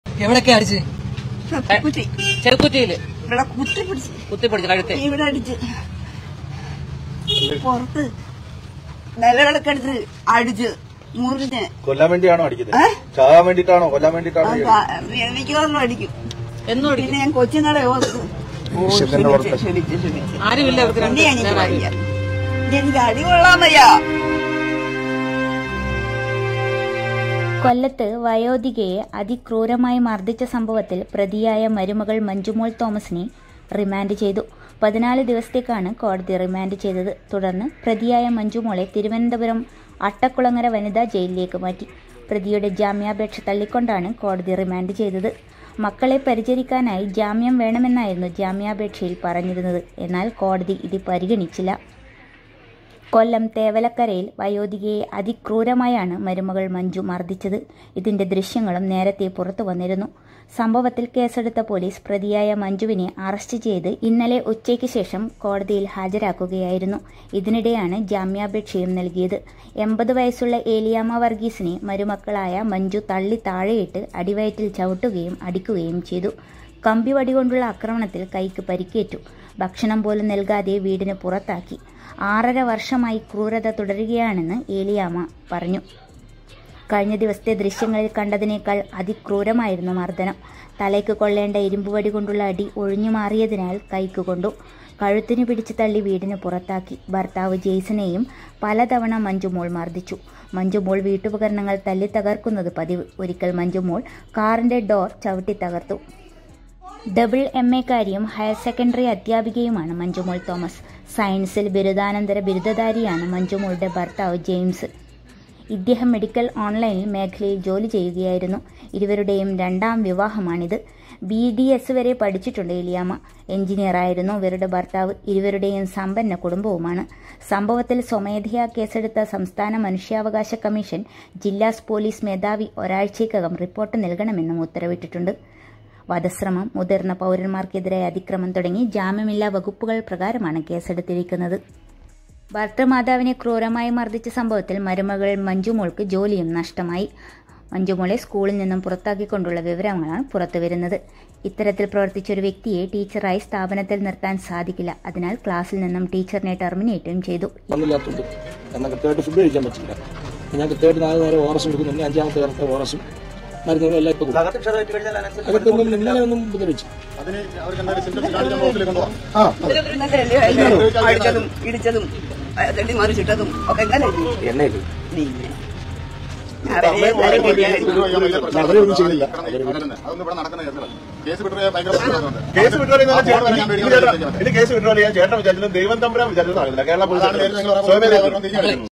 വയോധികയെ അതിക്രൂരമായി മർദിച്ച സംഭവത്തിൽ മഞ്ജുമോൾ റിമാൻഡിൽ.അമ്മായിയമ്മയെ തല്ലിയ മരുമോളുടെജോലിയുംപോയി
കണ്ണില്ലാത്ത ക്രൂരതയ്ക്ക് ജാമ്യമില്ല; വയോധികയെ അതിക്രൂരമായി മർദിച്ച സംഭവത്തിൽ മരുമകൾ മഞ്ജുമോൾ റിമാൻഡിൽ..
മക്കളെ പരിചരിക്കാനായി ജാമ്യം വേണമെന്നായിരുന്നു ജാമ്യാപേക്ഷയിൽ
പറഞ്ഞിരുന്നത്. എന്നാൽ, കോടതി ഇത് പരിഗണിച്ചില്ല.